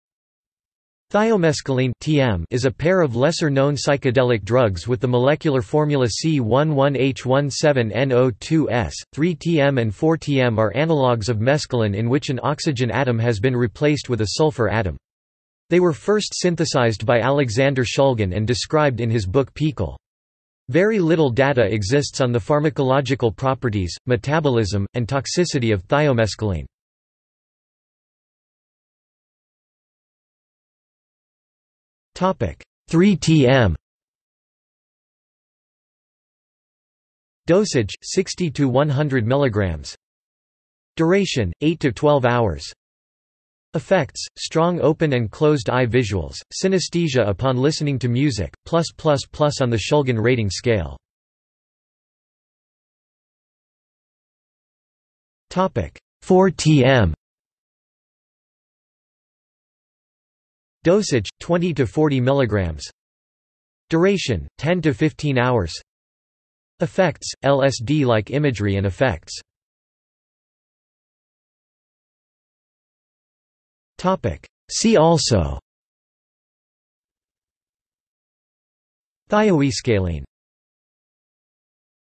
Thiomescaline | Wikipedia audio article
This is an audio version of the Wikipedia Article:
Thiomescaline

00:00:28 1 3-TM
00:00:51 2 4-TM
00:01:10 3 See also

- Socrates

SUMMARY
Thiomescaline (TM) is a pair of lesser-known psychedelic drugs with the molecular formula C11H17NO2S. 3-TM and 4-TM are analogs of mescaline in which an oxygen atom has been replaced with a sulfur atom. They were first synthesized by Alexander Shulgin and described in his book PiHKAL. Very little data exists on the pharmacological properties, metabolism, and toxicity of thiomescaline.